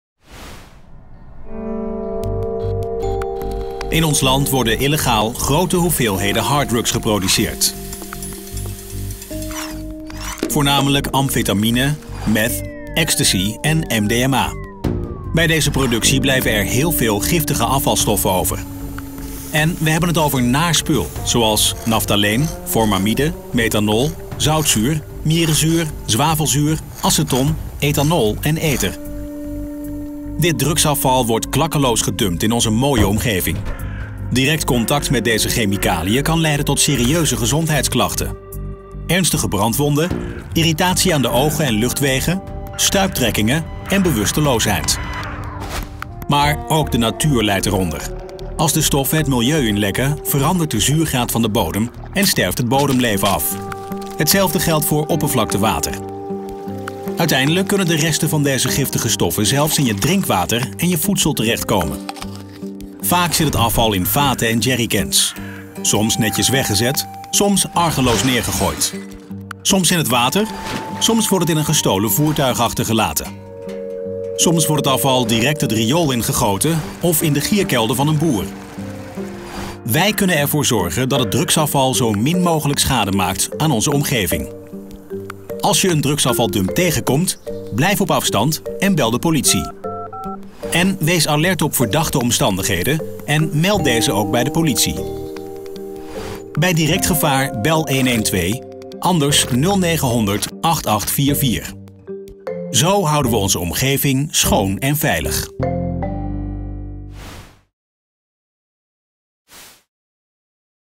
Drugsafvaldumping in Nederland - Gedumpt drugsafval is schadelijk
Creative Beards maakte een animatie over drugsafvaldrumping in Nederland. Bij de productie van drugs blijven veel afvalstoffen over die klakkeloos worden gedumpt. Gedumpt drugsafval is schadelijk, mensen en natuur leiden eronder. Verdachte activiteit of drugsafval gevonden? Meld het!